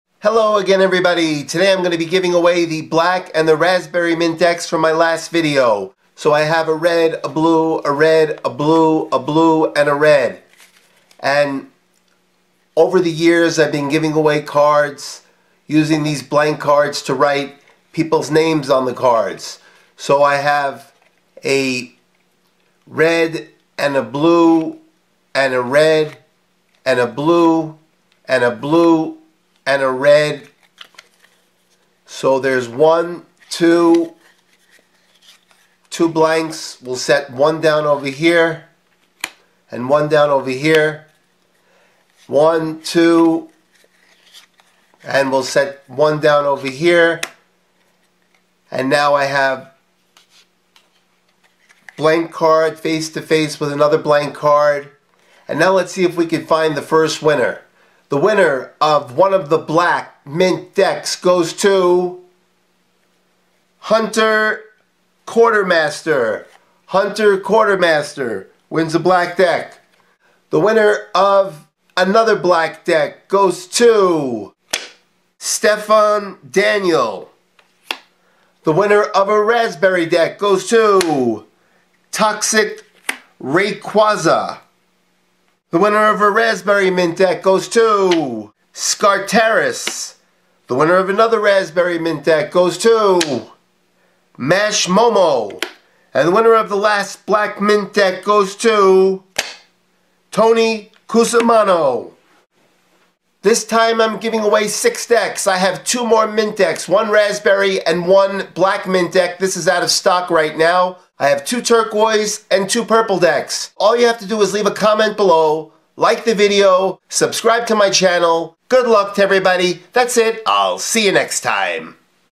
Mint Deck Winners Card Trick
Mint Deck winners revealed in a card trick. It's a clever gimmicked card trick. Would you like to learn how to do it?
I will be announcing winners in my next video.